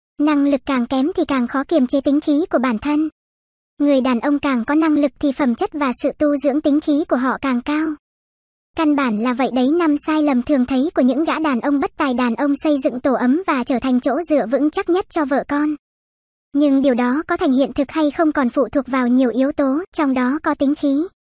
5 sai lầm thường thấy của những gã đàn ông bất tài
5 sai lầm thường thấy của những gã đàn ông bất tài

Đàn ông xây dựng tổ ấm và trở thành chỗ dựa vững chắc nhất cho vợ con. Nhưng điều đó có thành hiện thực hay không còn phụ thuộc vào nhiều yếu tố, trong đó có tính khí.

Năng lực càng kém thì càng khó kiềm chế tính khí của bản thân. Người đàn ông càng có năng lực thì phẩm chất và sự tu dưỡng tính khí của họ càng cao. Dù ở bên ngoài có chứng kiến bao điều sai trái, gặp phải bao nhiêu gian khổ thì những nóng nảy, oán hận của anh ta cũng sẽ dập tắt hết khi trở về bên những người mình yêu thương. Họ thậm chí còn không mang những bực dọc về nhà phàn nàn trước mặt người phụ nữ của mình chứ đừng nói đến việc mắng mỏ họ. Ngược lại, những người đàn ông kém năng lực lại rất khó kiềm chế bản thân và thường mắc phải sai lầm. Hãy xem đâu là những sai lầm sẽ lặp lại ở những người này:

Không tôn trọng vợ

Người ta thường nói “đàn ông bất tài thường bạc bẽo” là có lý do cả.

Một bên, làm chút công chuyện bên ngoài, cũng chẳng phải ông này ông nọ nhưng khi về nhà thì tiếng to tiếng lớn quát tháo vợ con, buông những lời lẽ thiếu tôn trọng “không có tôi thì cái nhà này ra đường”, “cơm ăn, áo mặc hàng ngày của cô là tôi cho cả”.

Một bên, làm công to chuyện lớn bên ngoài, uy phong lẫm liệt nhưng khi trở về với vai trò người chồng, người cha thì sẵn sàng cởi bỏ chiếc áo bệ vệ để độ lượng với con và tôn trọng từng công việc nhỏ nhặt của vợ.

Người đàn ông này không lấy việc ở trên cơ vợ mình để làm niềm tự hào của bản thân, thậm chí sẵn sàng nhún nhường vợ chỉ để tạo ra sự ngọt ngào cho thế giới hai người. Vì sao họ có thể làm được vậy? Bởi vì một người đàn ông có năng lực hoàn toàn có thể học cách cân bằng cuộc sống của mình. Họ vừa có thể đảm đương tốt công việc kinh doanh của mình vừa đem lại hạnh phúc cho những người thân yêu của mình. Anh ấy đủ sáng suốt để phân tách được sự khác biệt giữa gia đình và bên ngoài, giữa bất biến và hoạt biến trong cuộc sống.

Ít quan tâm đến gia đình

Ý nghĩa của công việc đến từ gia đình. Nhưng thường thì một số cánh mày râu lại không thể nói được thứ tự ưu tiên này. Khi về nhà, họ sẽ là chồng và là cấp trên; không chỉ giao cho vợ những công việc nhà tẻ nhạt mà ngay cả con cái cũng không muốn dành thời gian chăm lo.

Những người đàn ông khéo léo lại khác. Họ không chỉ biết vận hành guồng máy công việc mà họ còn hiểu trách nhiệm làm chồng, làm cha của mình đặt ở vị trí nào trong cuộc sống. Một người đàn ông quan tâm đến con cái và vợ mình, chu toàn hoàn thành trách nhiệm cách cẩn thận mới được coi là người đàn ông trụ cột của gia đình.

Dùng lời phàn nàn để giải tỏa

Một người đàn ông cân bằng luôn tự tin và sẽ không phàn nàn về vợ như một bao cát trút mọi giận dữ, bực tức hoặc lo âu. Dù sự nghiệp có thăng trầm thế nào, họ cũng hiểu vợ con là chỗ dựa vững chắc của mình và luôn nỗ lực hết mình vì gia đình.

Điều này trái ngược hoàn toàn với người luôn thích và chọn cách phàn nàn vợ, xem người đồng cam với mình như một cái thùng rác. Chưa kể người này còn năm lần bảy lượt tìm cách thoát ra khỏi thế giới của người chồng, của người cha để tìm đến thế giới của riêng mình ở bên ngoài. Nhưng khi mọi chuyện vỡ lở lại tìm cách đổ hết mọi lỗi lầm lên đầu vợ như một lý do để trốn thoát.

Ít bao dung

Đàn ông khéo léo, có năng lực sẽ không để bản thân bị vướng bận bởi những rắc rối vụn vặt trong cuộc sống. Đối với những cuộc cãi vã nhỏ nhặt trong cuộc sống, họ sẵn sàng là người nhường nhịn, ôm vợ để xin làm hòa trước. Họ sẽ dùng trí óc rộng mở để thích ứng với sự khéo léo và ý chí của vợ. Trong công việc, họ đầu óc biết nhìn xa trông rộng, biết bao quát thống lĩnh để hoạch định sự nghiệp và chăm chỉ làm việc để xây dựng một gia đình tươi đẹp trong tương lai.

Việc tung hoành bên ngoài là tùy thuộc vào đàn ông và đàn ông cần phải thể hiện tham vọng và niềm kiêu hãnh mà họ nên có.

Nhưng ở nhà, đối mặt với người vợ đã dành mọi thứ cho bạn, năng lực tốt của một người đàn ông là sự dịu dàng và tình yêu thương lớn nhất để đáp lại.

Năng lực lớn nhất của một người chồng, người cha trong gia đình không phải là kiếm được bao nhiêu tiền hay mua bao nhiêu căn nhà, bao nhiêu chiếc xe. Cuộc đời chẳng ai biết trước được điều gì xảy ra. Người giàu có, người khôn ngoan một ngày nào đó thời vận sụp đổ rồi cũng chẳng tìm được ai đồng hành ngoài người bạn đời sắt son. Bởi vậy, quan trọng hơn hết, trong mái ấm và sau lưng người đàn ông luôn là người vợ yêu thương thật lòng, sẵn sàng vun đắp ấm áp cho hạnh phúc gia đình.